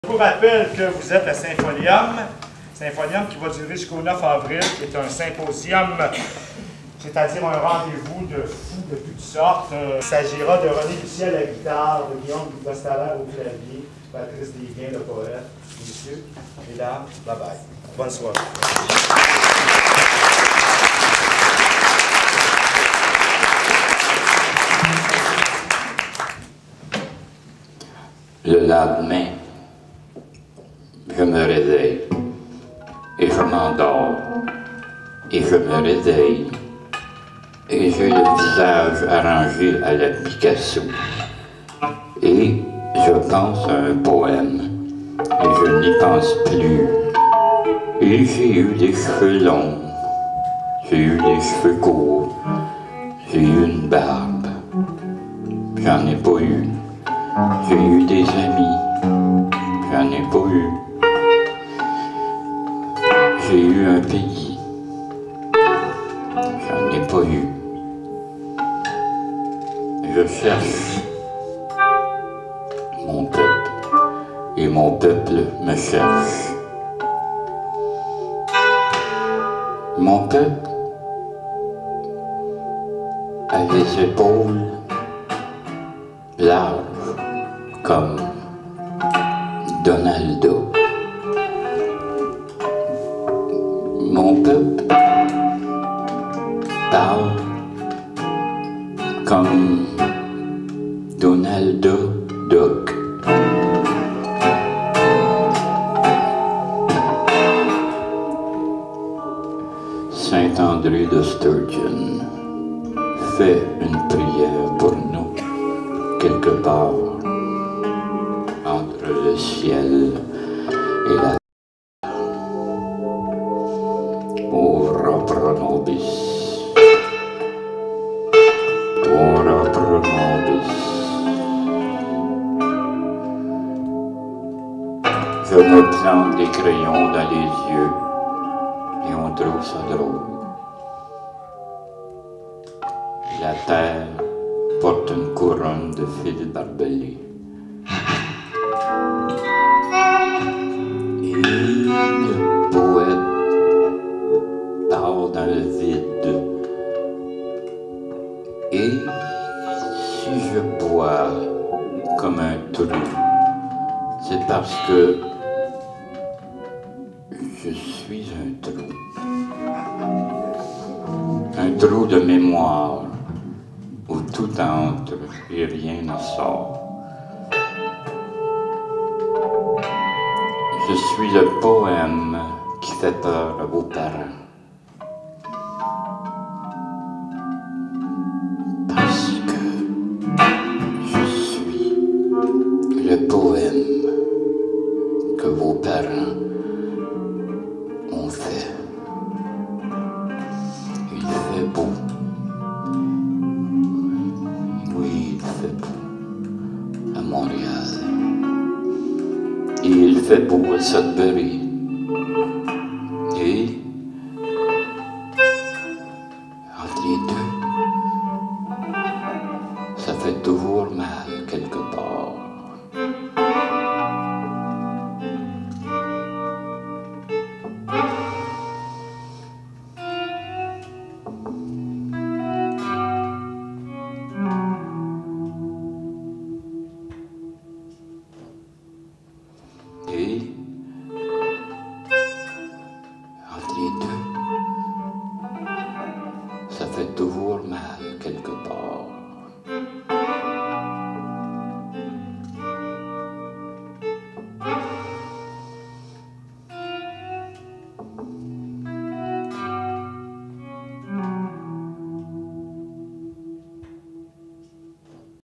Patrice Desbiens et les Moyens du Bord «1» Symfolium 2000
Patrice Desbiens et Les Moyens du Bord
avec Patrice Desbiens, René Lussier et Guillaume Dostaler
Dans le cadre du Symfolium 2000
3 avril 2000
Maison de la culture du Plateau Mont-Royal
Caméra et montage Coeurdanik
Productions Coeurdanik